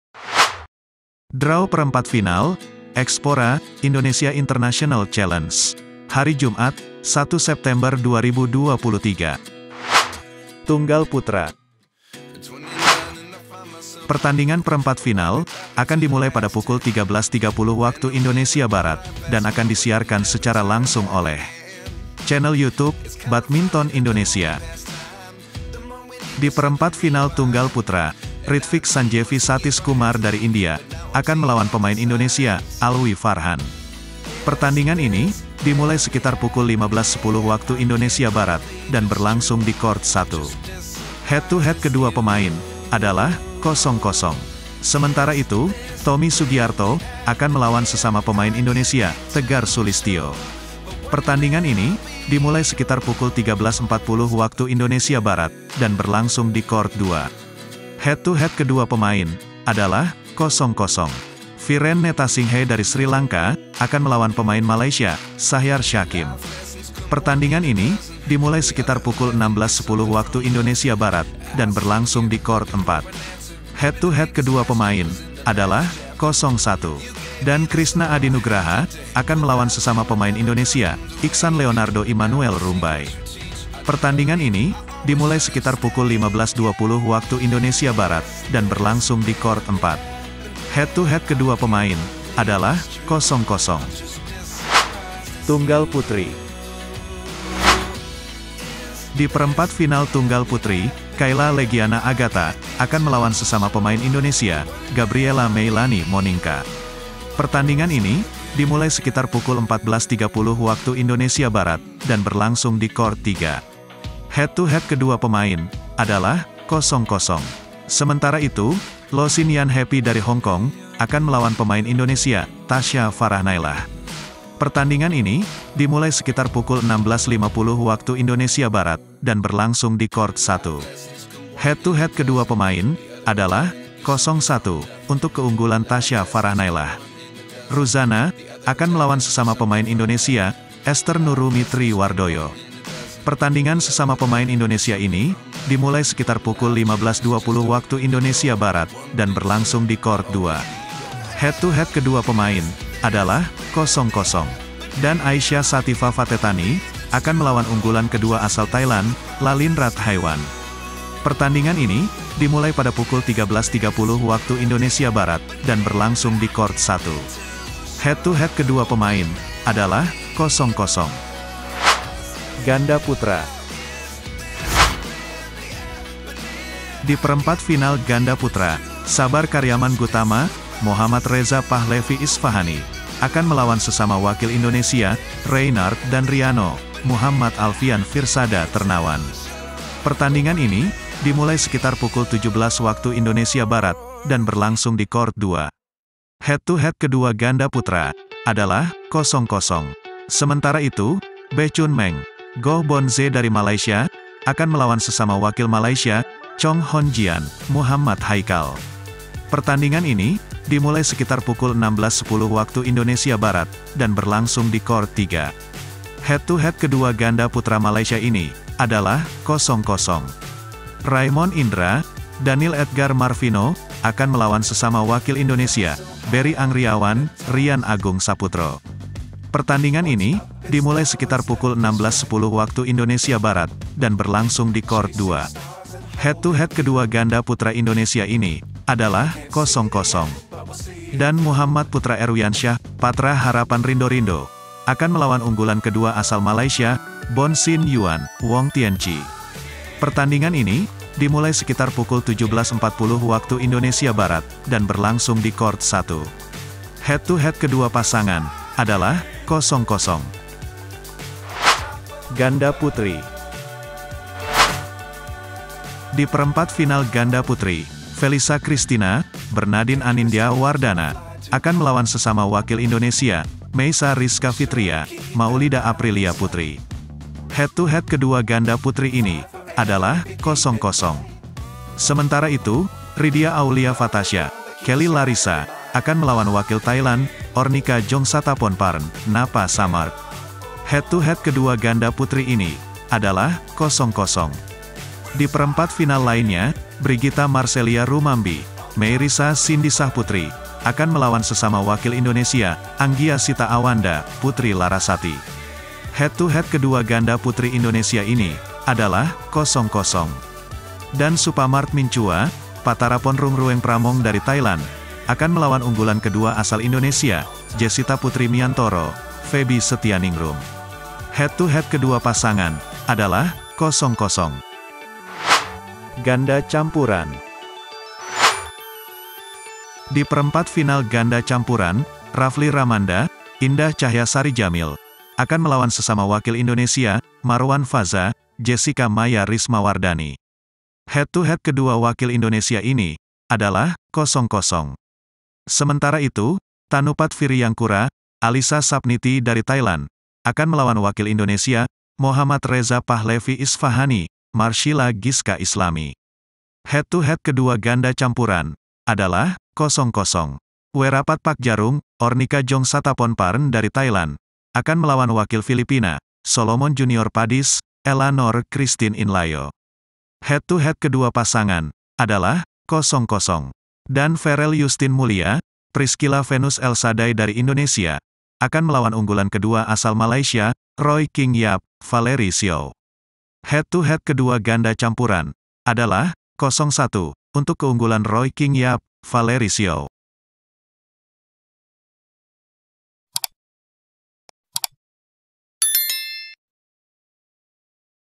Draw & Jadwal Perempat Final Xpora Indonesia International Challenge 2023 Live Badminton Indonesia
Draw & Jadwal Pertandingan Perempat Final Badminton Xpora Indonesia International Challenge 2023. Hari Ini Mulai Pukul 13:30 WIB Live Youtube Badminton Indonesia